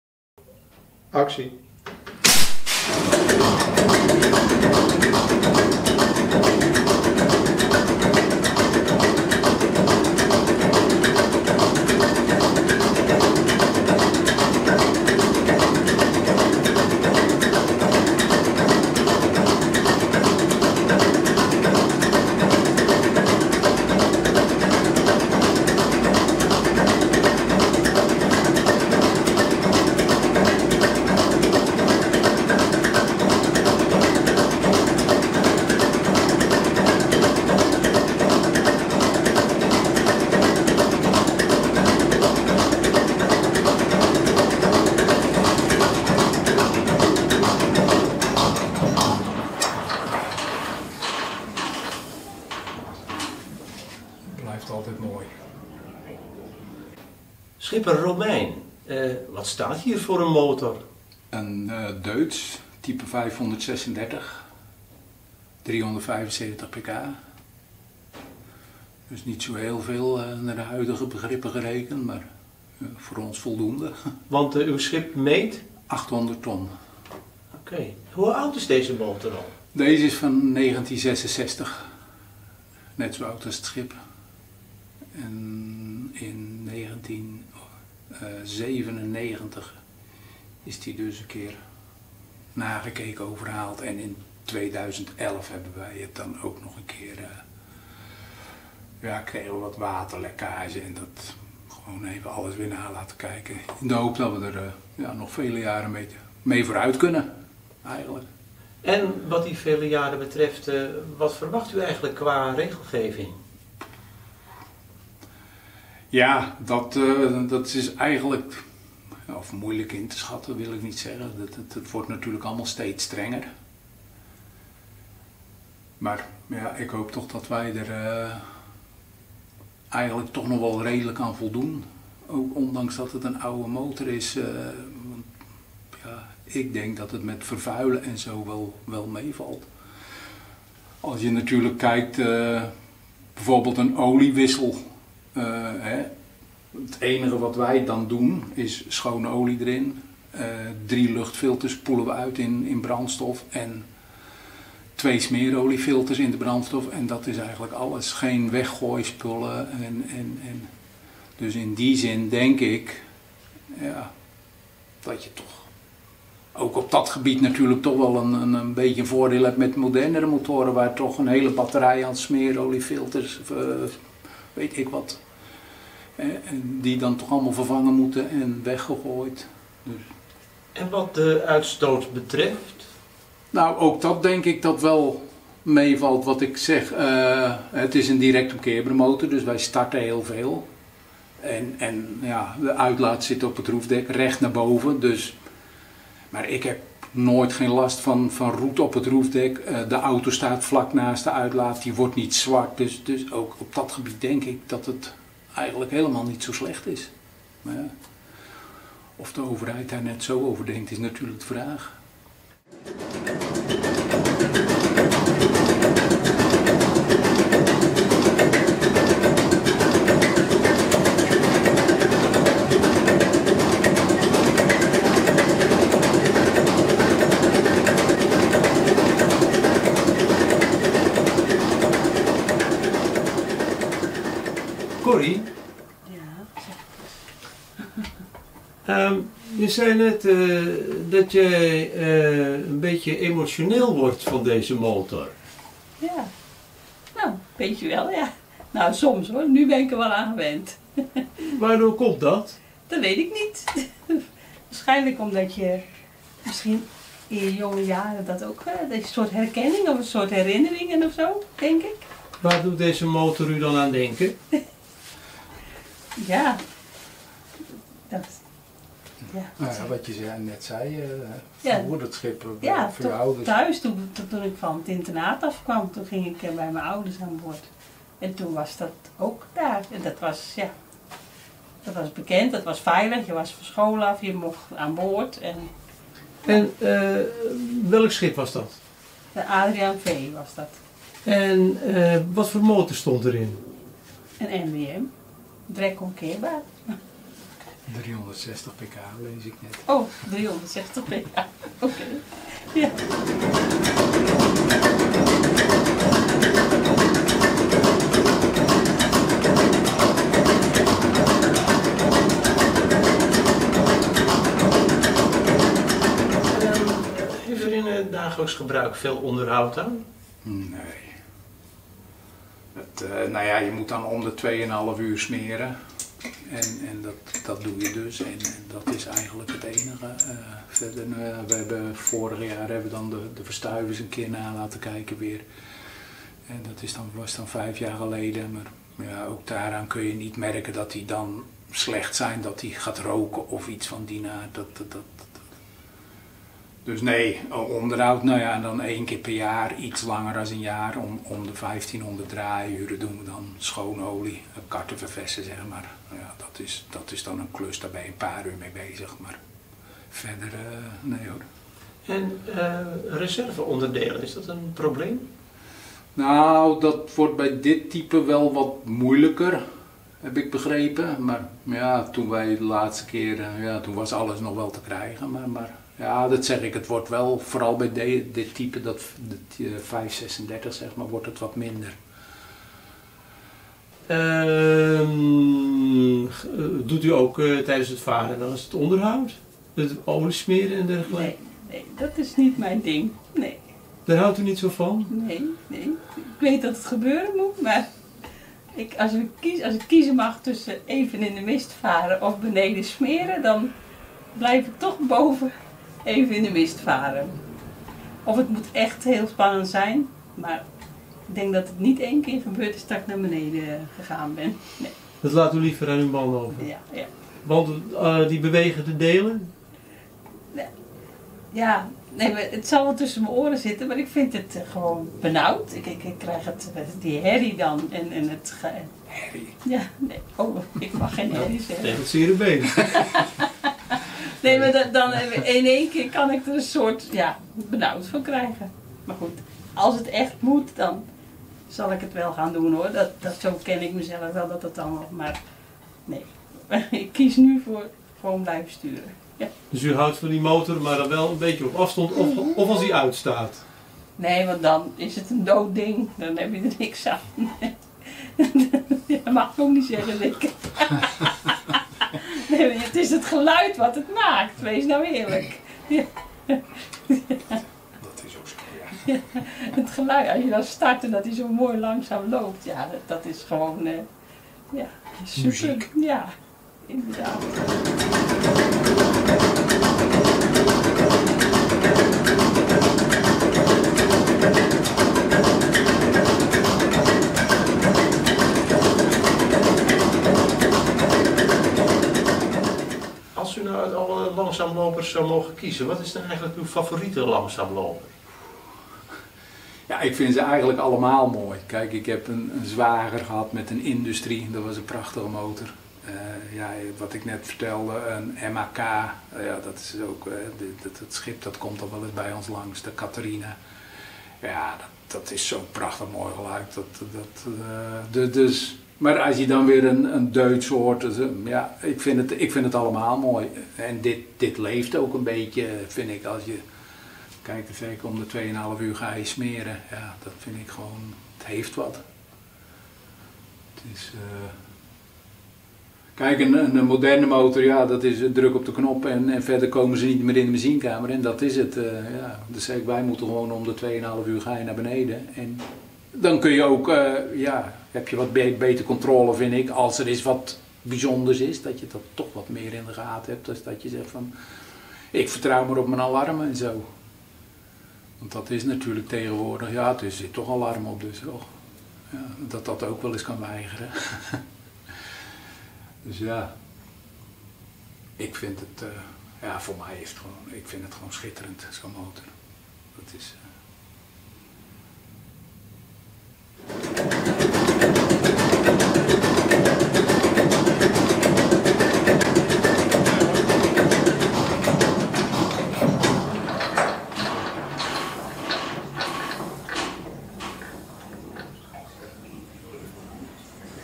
Loflied op de langzaamloper (2) - Deutz RV6M 536 op de West Europa
Op de als Wad- en Sontvaarder gebouwde 60-meter West Europa van Wout Romijn en Corrie Romijn-Verschoor staat een Deutz RV6M 536 die net als het schip stamt uit 1966. Voor Wout is het geluid van de langzaamloper muziek. Hij houdt oprecht van zijn motor - van alle langzaamlopers trouwens. ‘Ik vind het gewoon schitterend, zo’n motor.’ Corrie werd vroeger emotioneel van de Deutz, omdat hij haar aan de MWM op het schip van haar ouders deed denken. Maar het smeren tijdens de vaart laat ze toch liever aan haar man over; ze blijft liever in de mist achter het stuur staan dan af te dalen in de machinekamer. Wout somt een aantal aspecten op waarin zijn oude langzaamloper minder vervuilend is dan een moderne snelloper. Ook ziet hij een voordeel in het om de twee en een halfuur te moeten smeren. ‘Dan heb je betere controle, vind ik. Een alarm kan ook weigeren.’